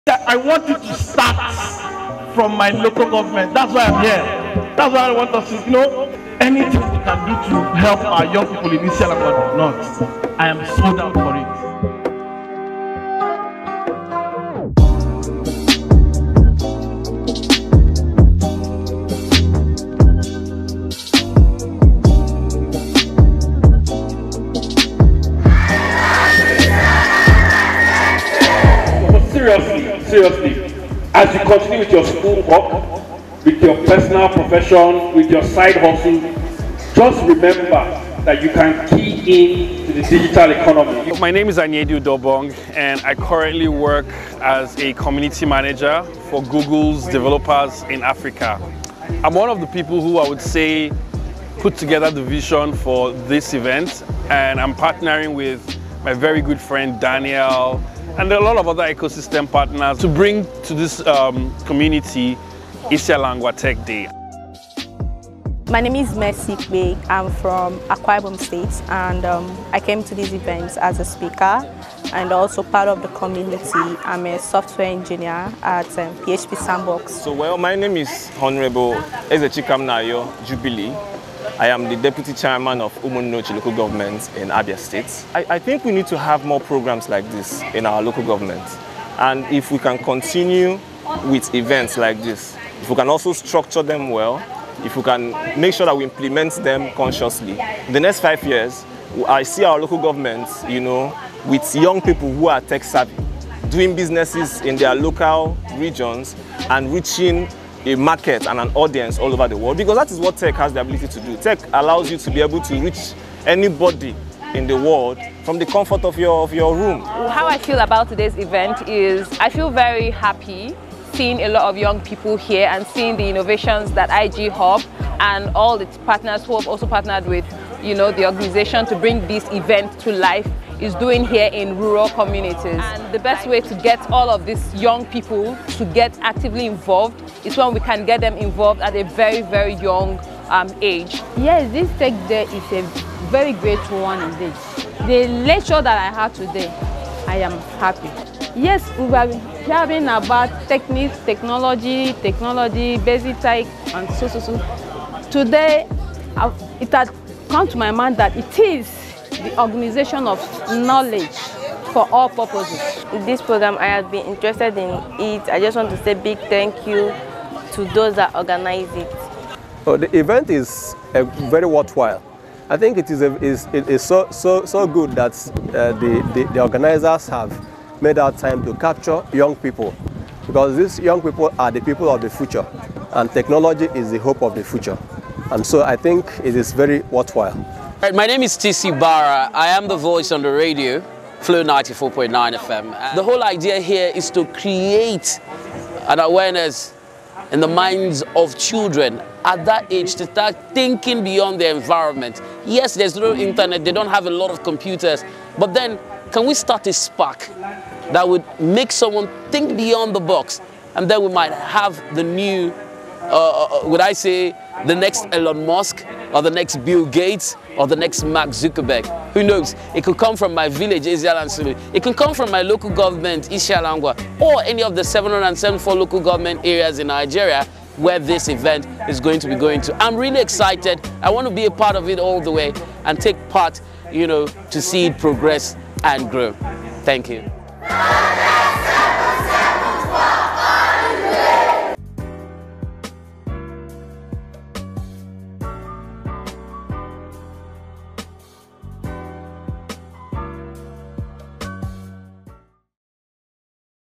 Isiala Ngwa Tech Day 2022
As part of our commitment to take technology to Rural communities we rolled out Project 774 in partnership with some friends in the tech space and my Local Government was one of the first beneficiaries in 2022